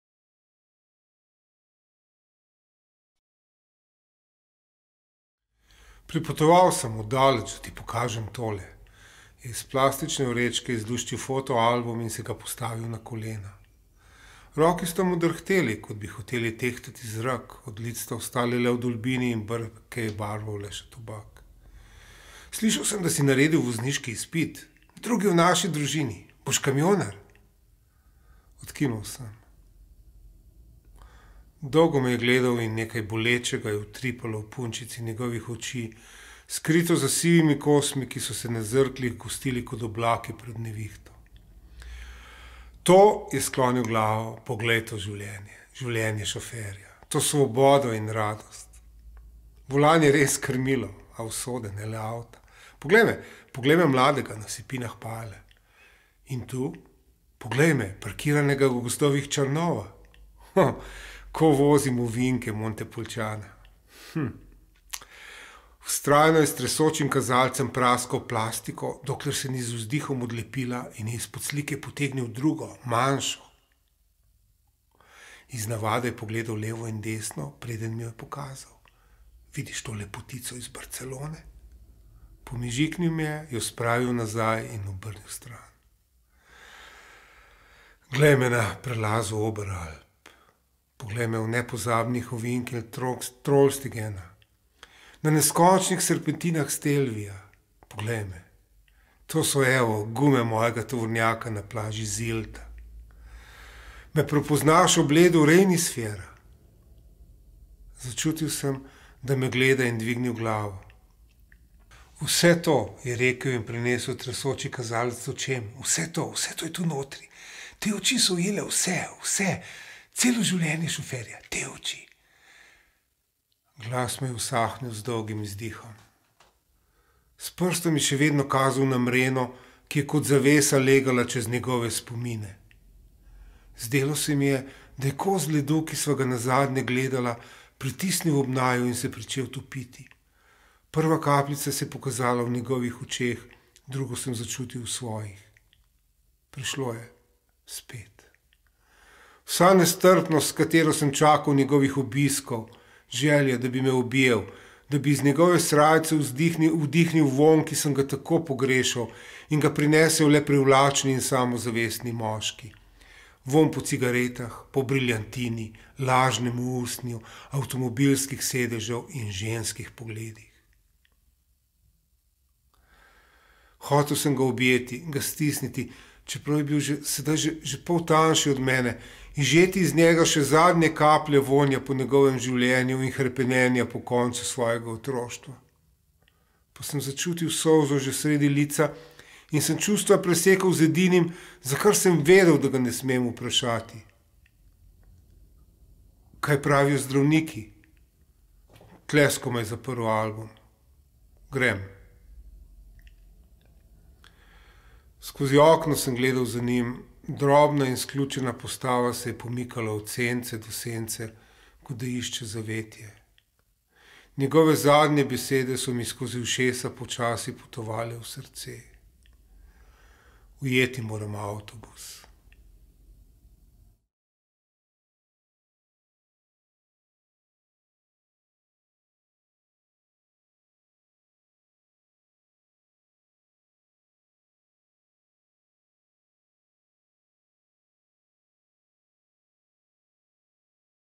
Stric Vinko, kratka zgodba iz romana Otroštvo
Otroštvo, avtobiografski roman v izmišljenih zgodbah.
Goga, 2015.
Miha Mazzini, režija in pripovedovanje
Tomaž Grdin, kamera in montaža